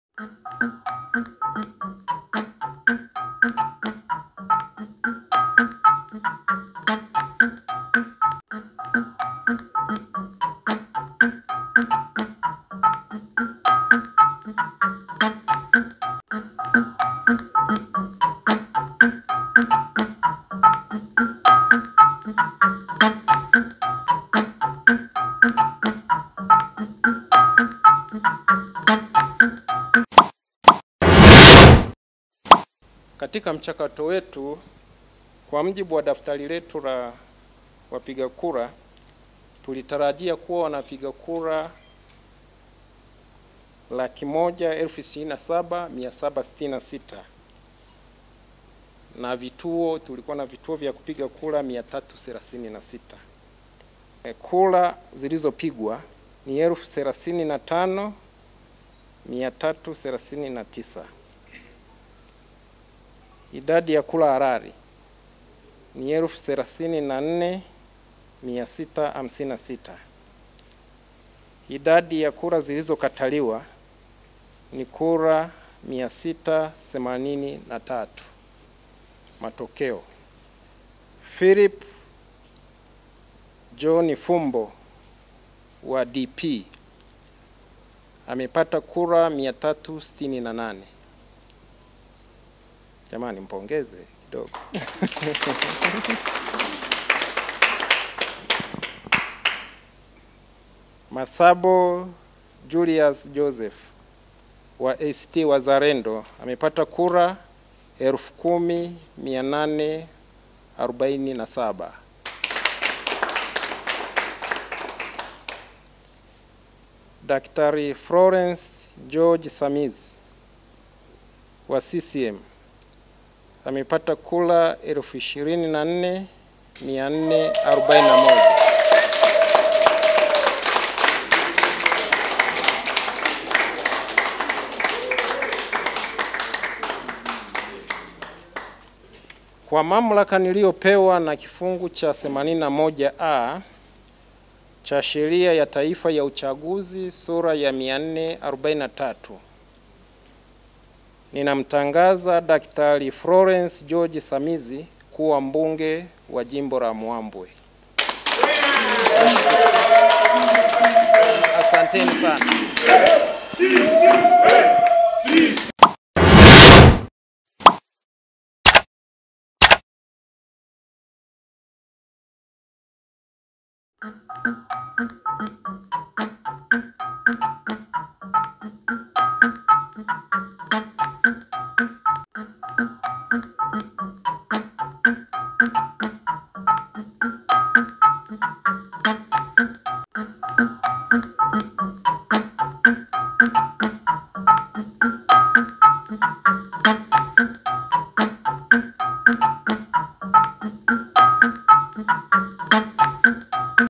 Hivi ndivyo matokeo ya uchaguzi jimbo la Muhambwe yalivyotangazwa, CCM waibuka kidedea
Msimamizi wa uchaguzi katika jimbo la Muhambwe mkoani Kigoma amemtangaza Dk Florence Samizi (CCM) kuwa mshindi wa uchaguzi mdogo uliofanyika katika jimbo hilo Mei 16 mwaka huu.

Uchaguzi huo mdogo umefanyika  kutokana na aliyekuwa mbunge wa jimbo hilo Atashasta Nditiye kufariki hapo Februari mwaka huu.

Akitangaza matokeo hayo Msimamizi wa Uchaguzi katika jimbo la Muhambwe, Diocles Rutema alisema katika uchaguzi huo, Mgombea ubunge kwa tiketi ya Chama Cha Mapinduzi (CCM) Dk Florence aliibuka na ushindi kwa kupata kura 23,441 sawa na asilimia 68 ya kura zote halali zilizopigwa.